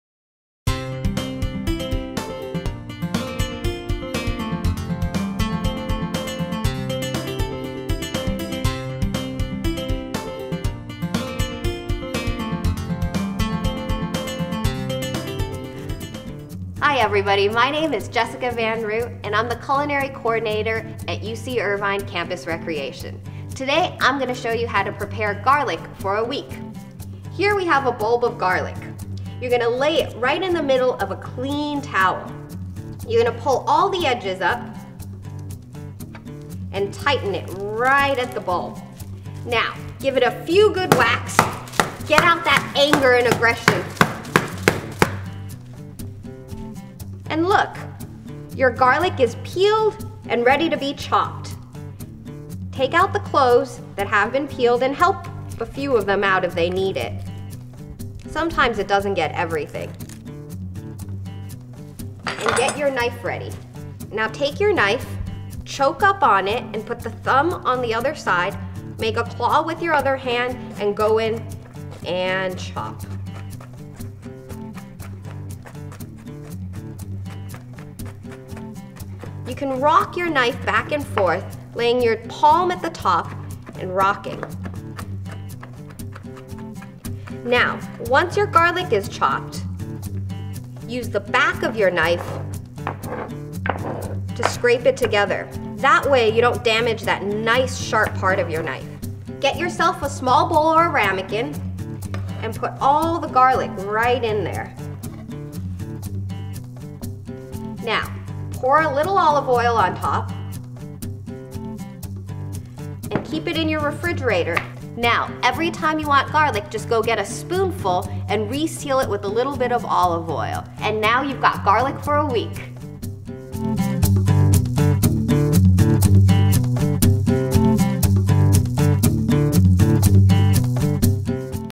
Cutting Garlic
Cooking with Chef Jessica

In this segment Chef Jessica shows you how cut and store garlic.